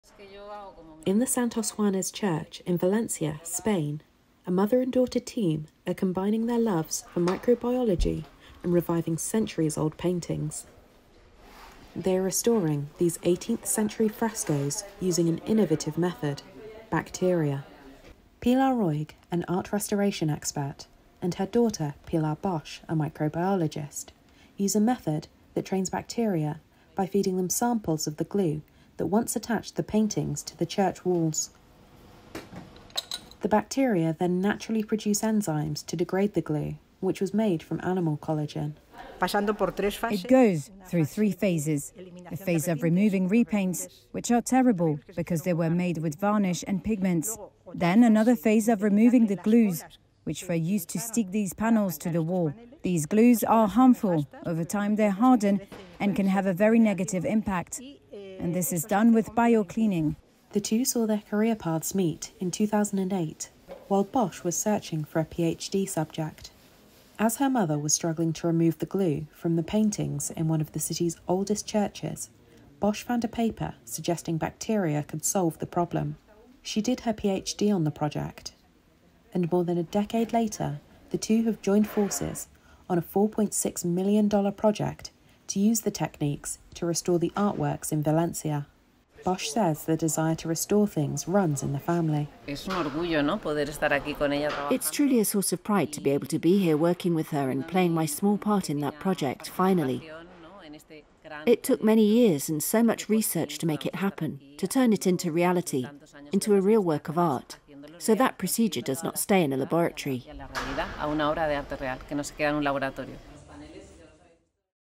Mother and daughter train bacteria to restore church frescoes | REUTERS
In Valencia’s Santos Juanes Church, Pilar Roig and her microbiologist daughter, Pilar Bosch, are restoring 18th-century frescoes using bacteria trained to remove glue from past restorations, combining their expertise in art and science.